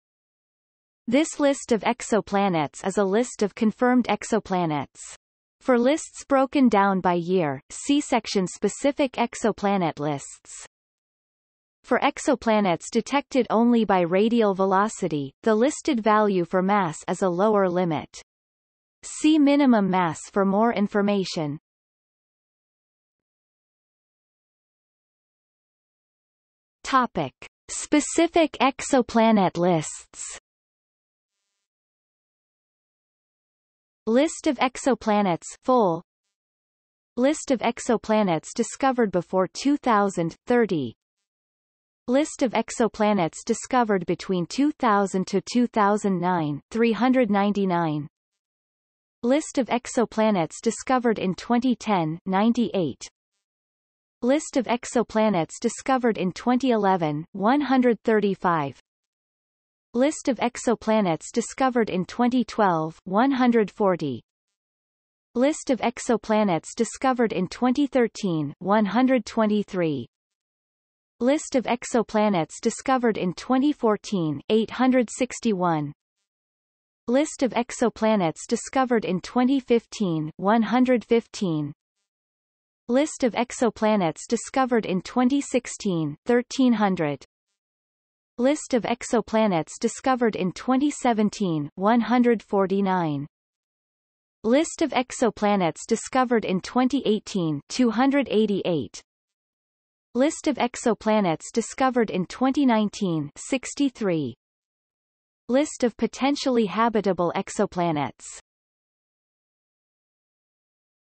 List of exoplanets (full) | Wikipedia audio article
00:00:23 Specific exoplanet lists

- Socrates

SUMMARY
This list of exoplanets is a list of confirmed exoplanets. For lists broken down by year, see § Specific exoplanet lists.
For exoplanets detected only by radial velocity, the listed value for mass is a lower limit. See Minimum mass for more information.